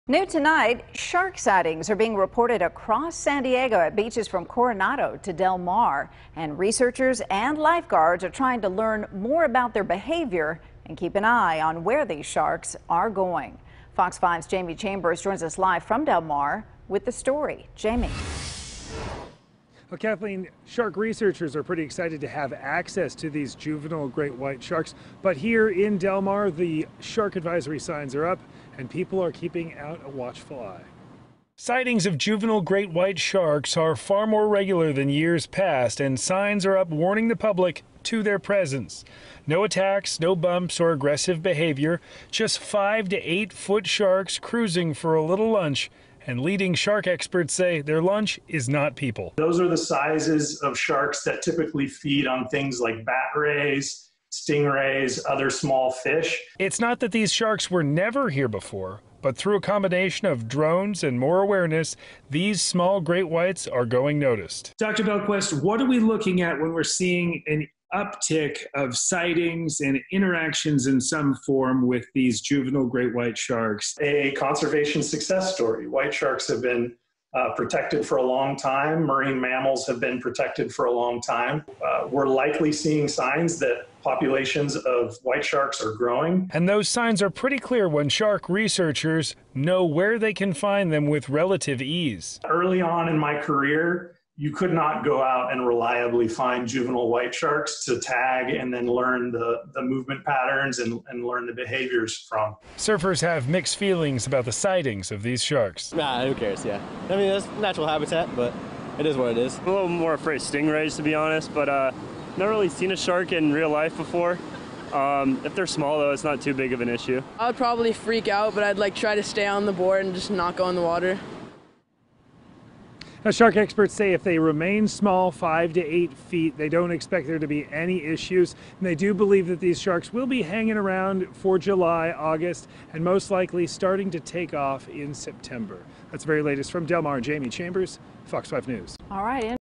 Spike In Shark Sightings
Great white shark sightings have been reported from Coronado to Del Mar in recent weeks, and researchers are scrambling the boats and tagging these five- to eight-foot juveniles.